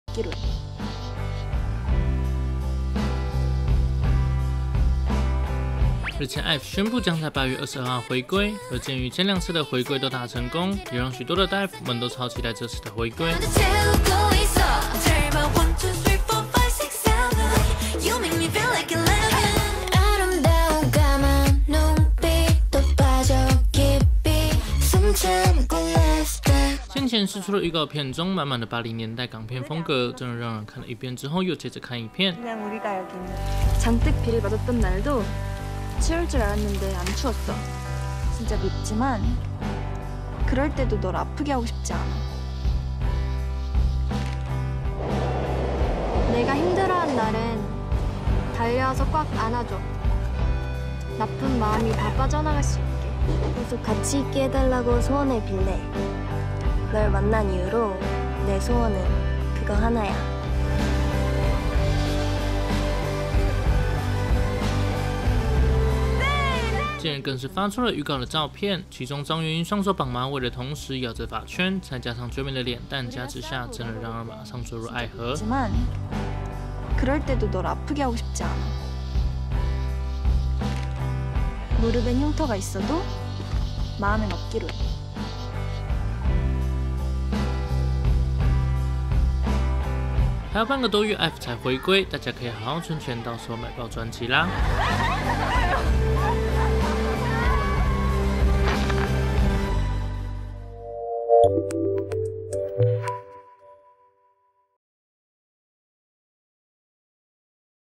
IVE回歸預告80年代港片感超唯美！張員瑛綁馬尾咬髮圈瞬間墜入愛河♥｜【IVE】
先前IVE釋出的預告片中滿滿的80年代港片風格，真的讓人看了一遍之後又接著看一遍!近日更是發出了預告的照片，其中張員瑛雙手綁馬尾的同時咬著髮圈，再加上絕美的臉蛋加持下真的讓人馬上墜入愛河!